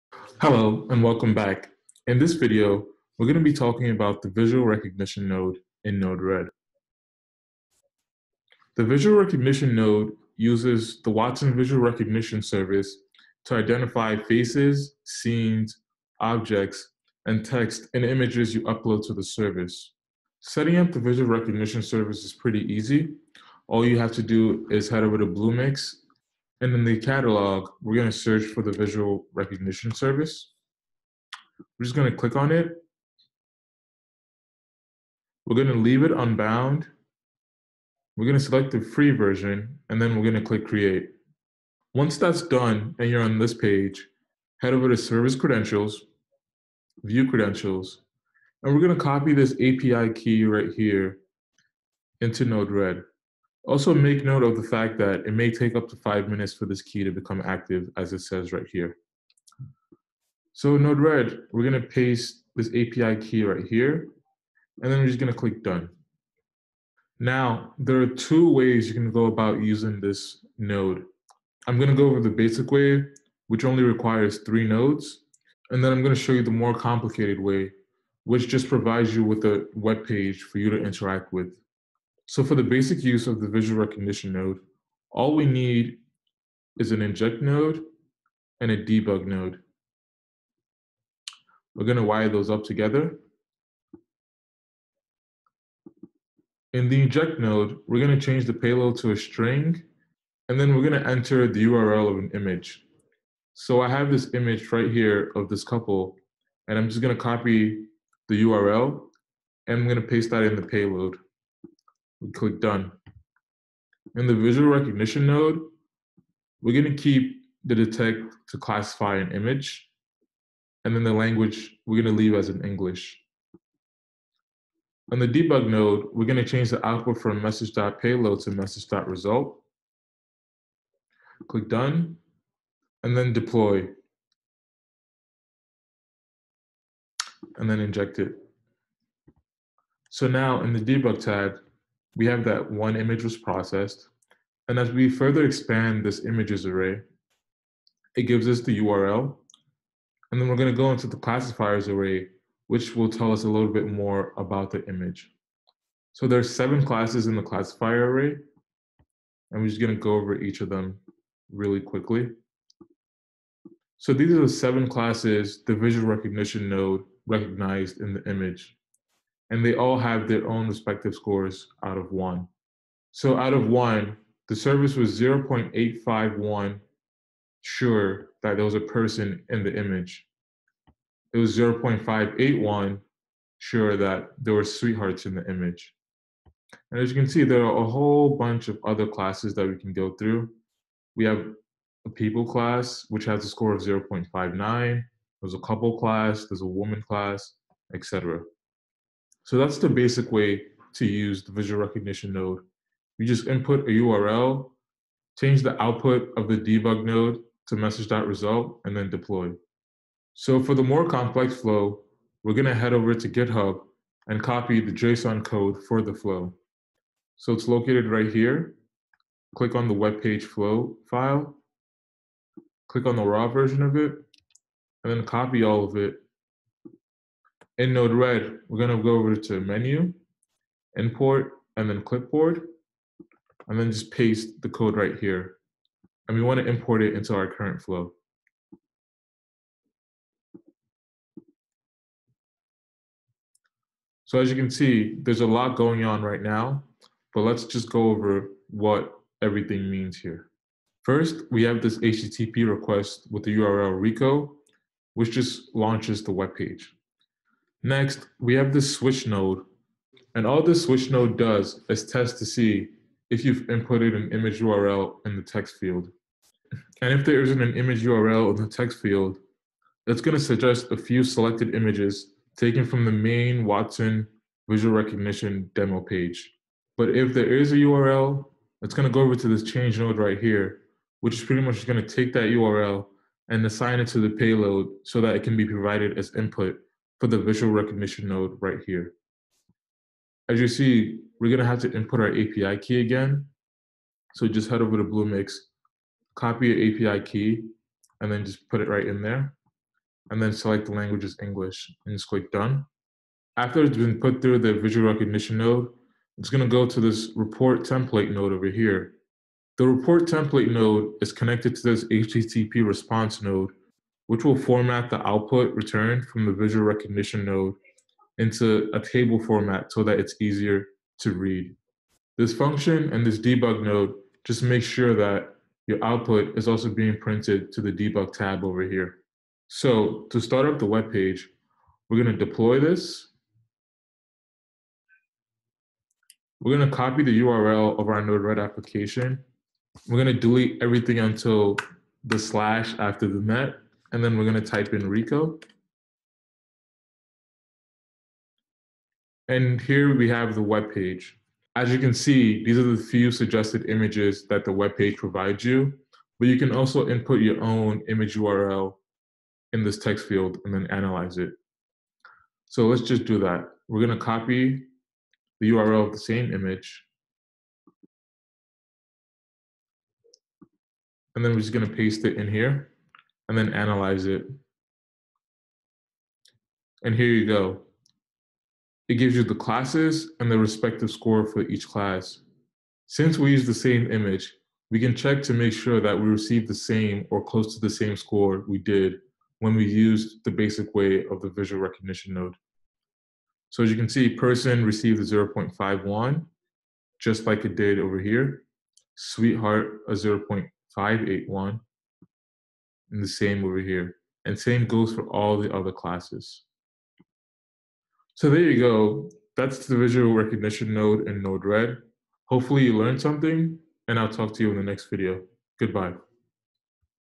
Node Red - Visual Recognition
This video will show you how to setup and use the visual recognition node in Node Red